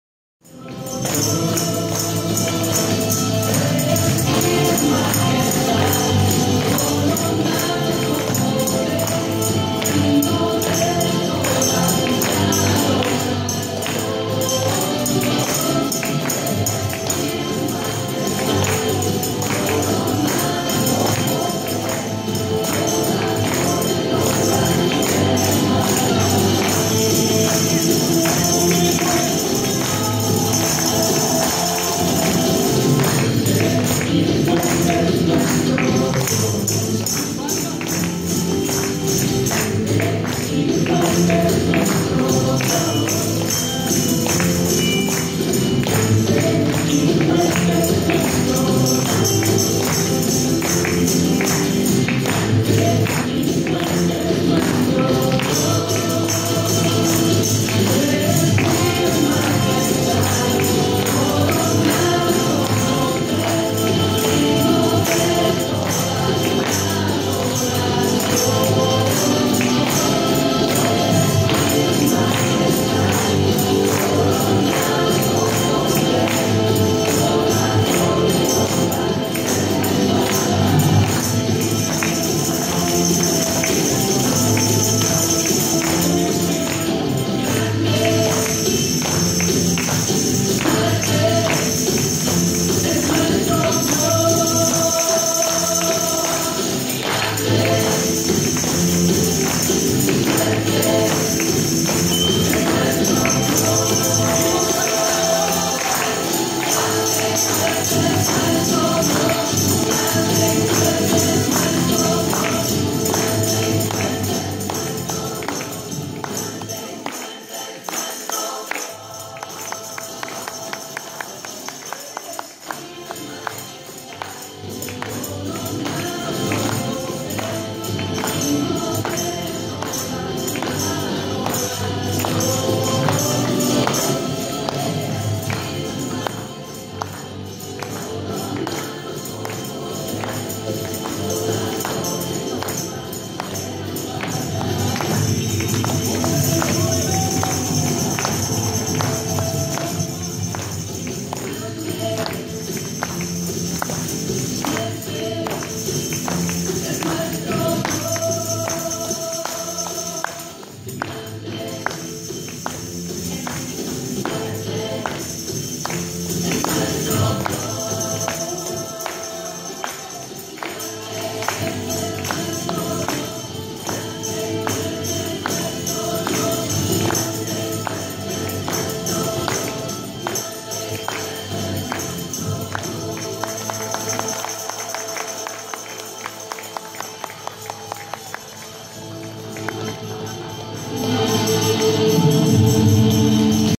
RUTY DANZANDO PARA EL SEÑOR.
RUTY ELIOT DANZANDO PARA NUESTRO REY JESUCRISTO. MINISTERIO CRISTIANO INTERNACIONAL LA CASA DE LAS CINCO FUENTES. WEBSTER CITY IOWA. PARA LAS NACIONES.